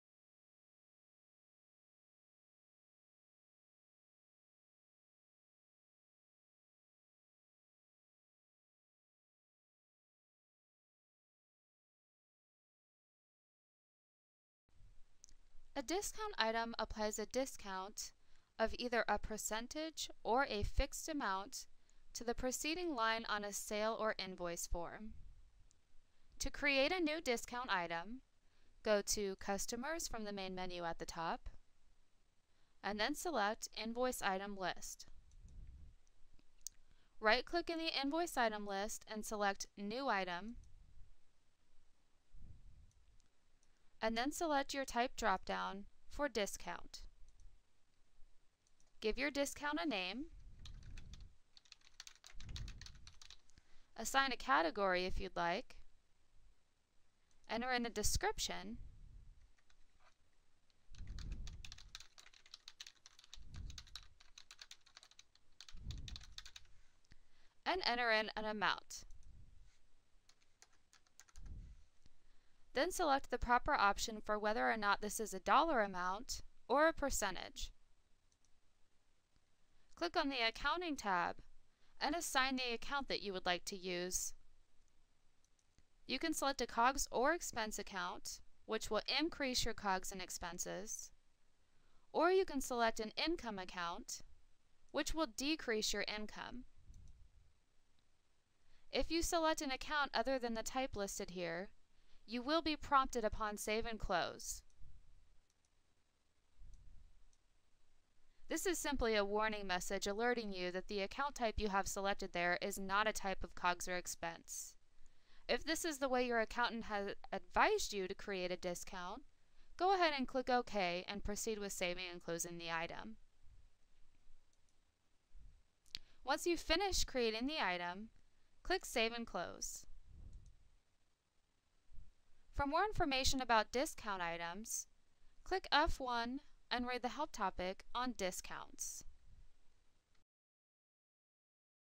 How to Use Discount Items in Total Office Manager
In this video, learn how to apply discounts as both percentages and fixed amounts to a sale or invoice in Total Office Manager